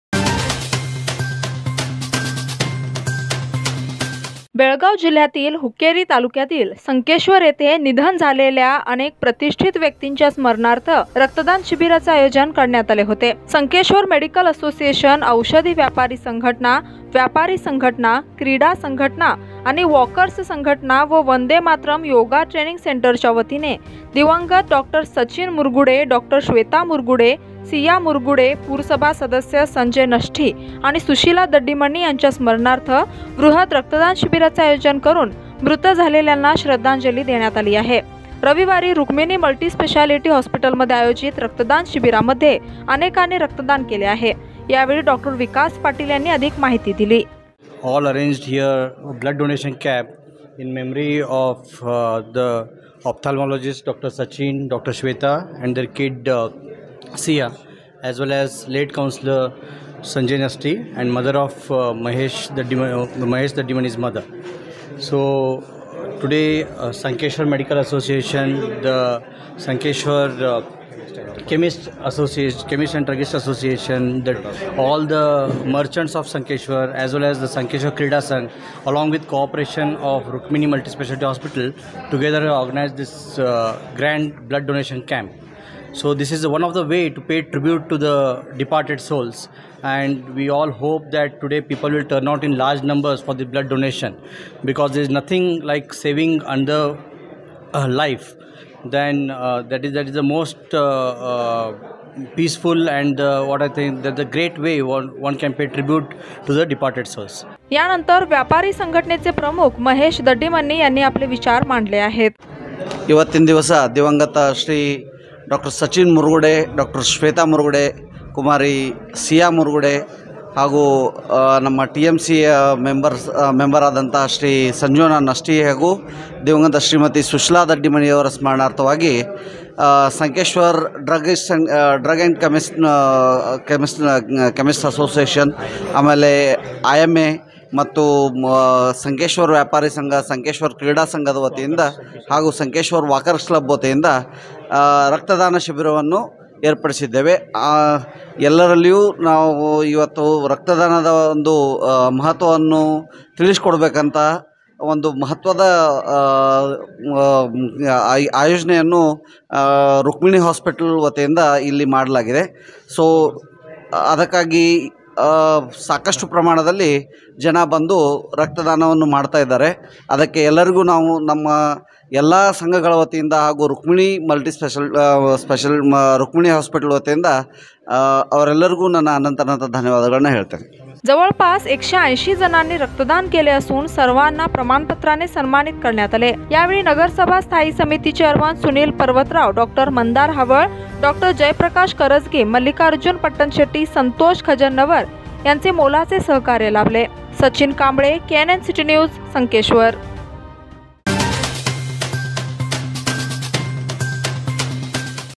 Sankeshwar Drug Merchant Association Organized Blood Donation Camp In Memory Of Dr. Sachin Murgude
Sankeshwar Drug Merchant Association Organized Blood Donation Camp In Memory Of Late Dr. Sachin Murgude